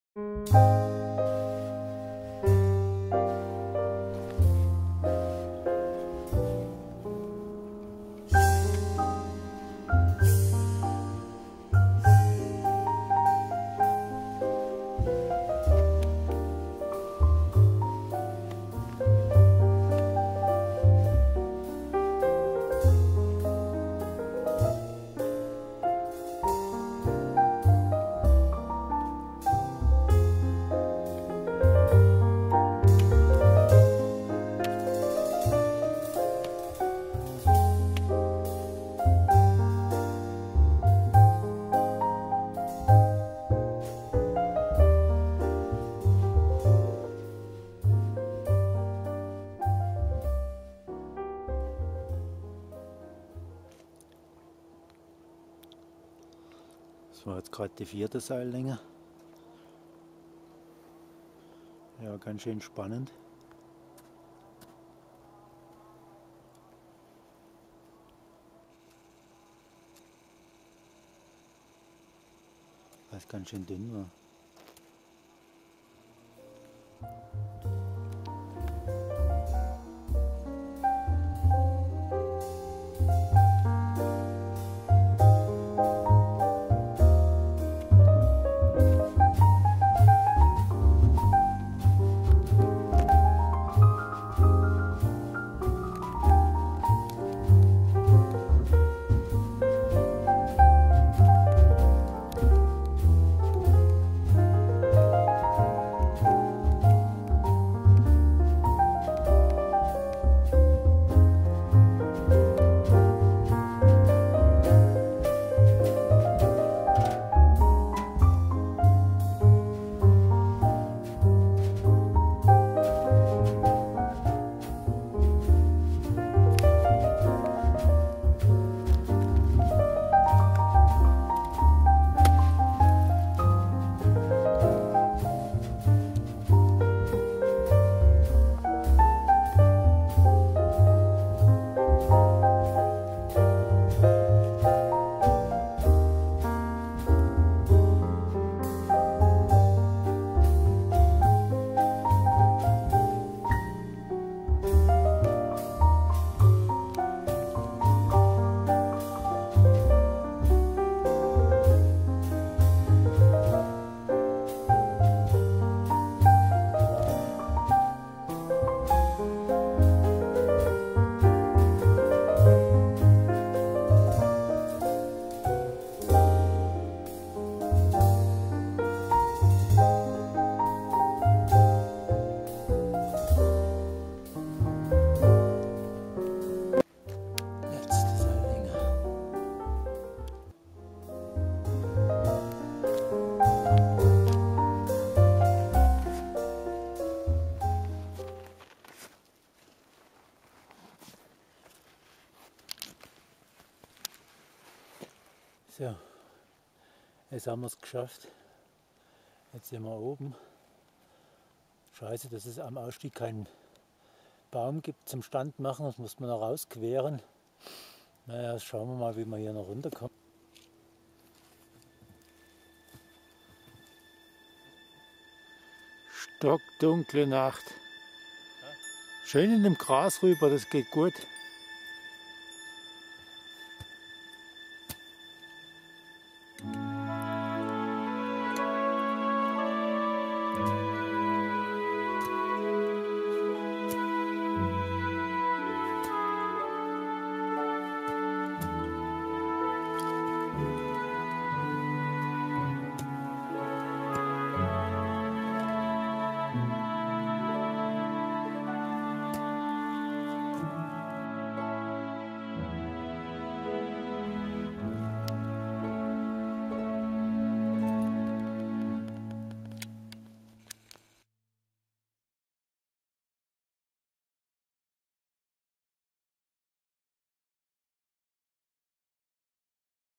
Ergotherapie
Erstbegehung von "Ergotherapie" im Brunnital